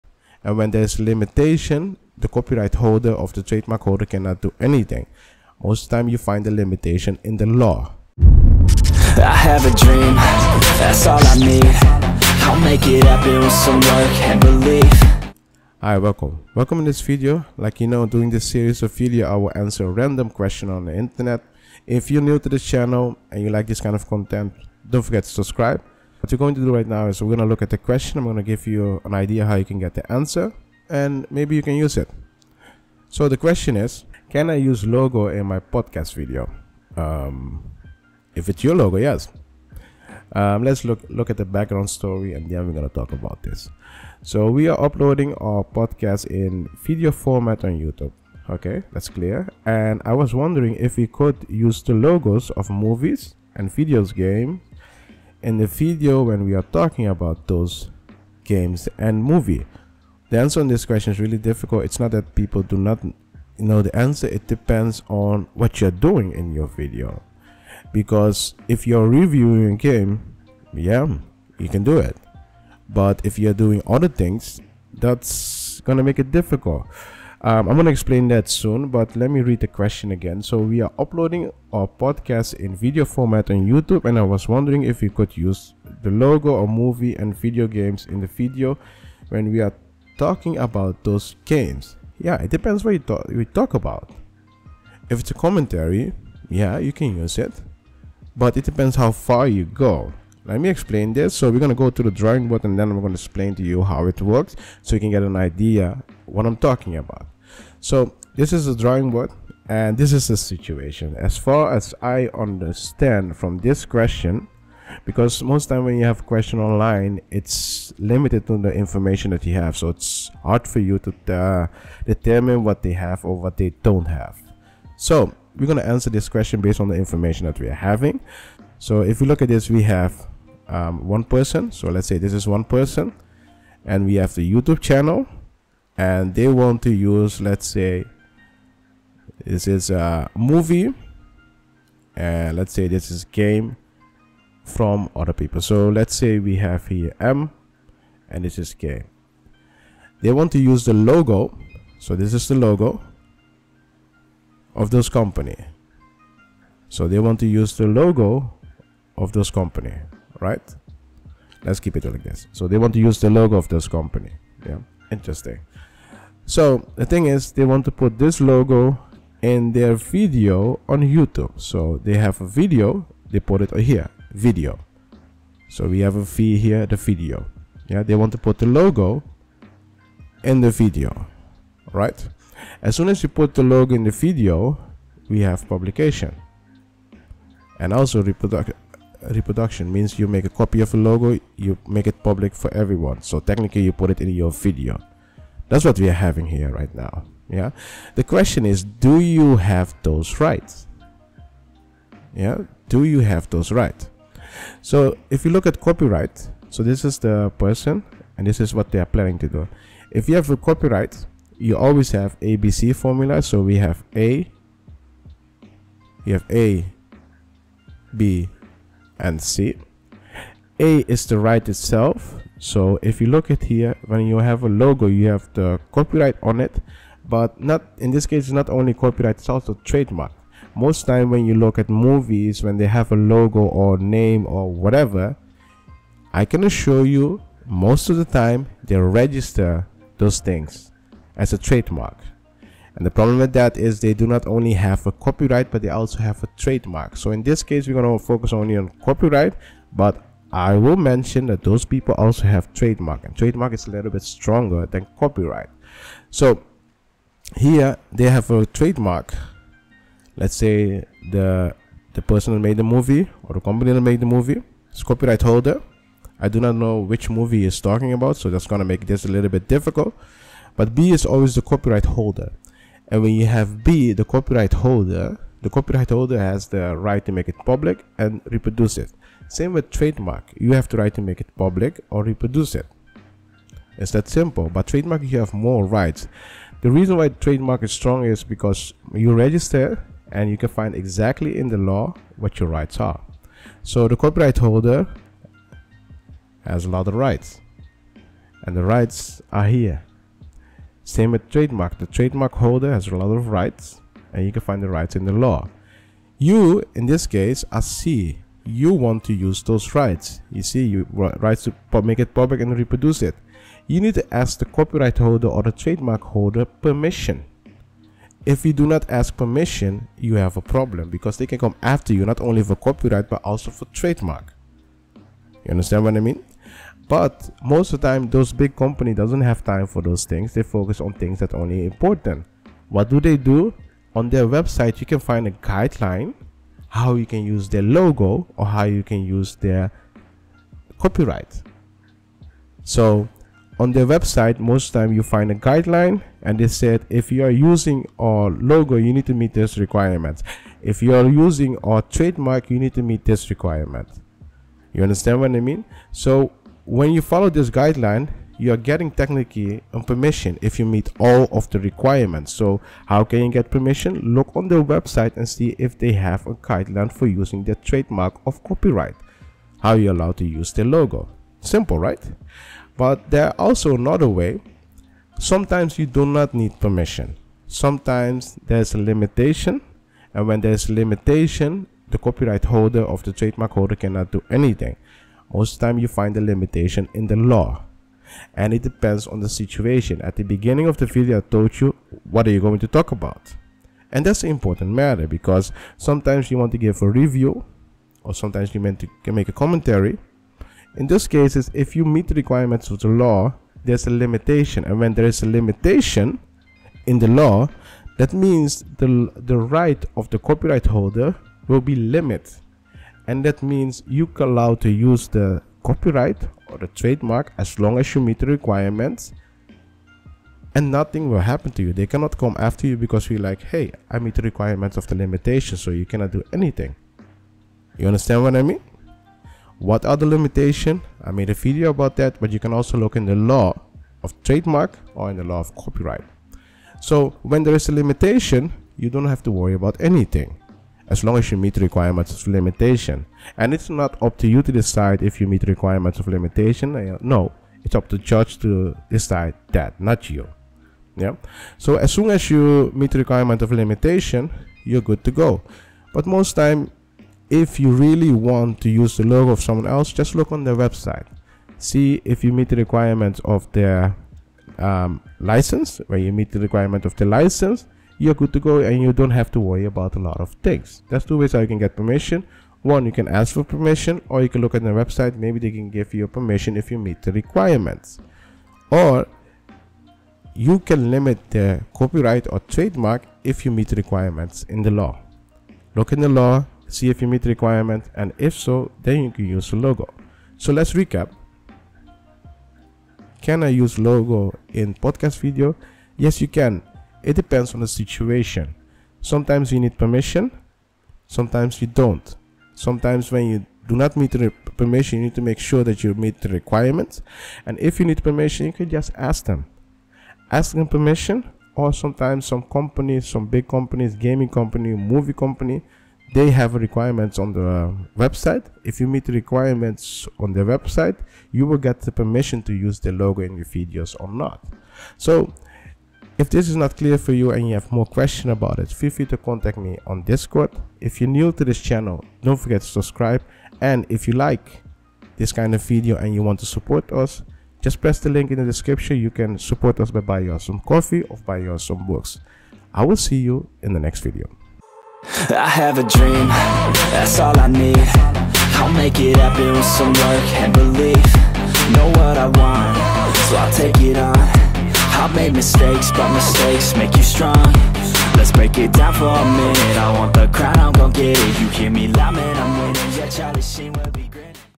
Can I Use Logo In Podcast Video? | Martisz Legal [ Internet Questions And Answers ]
In this video I will answer random questions on the internet. I will be discussing some things that you can use. These videos will be very helpful for entrepreneurs, business owners, and creators.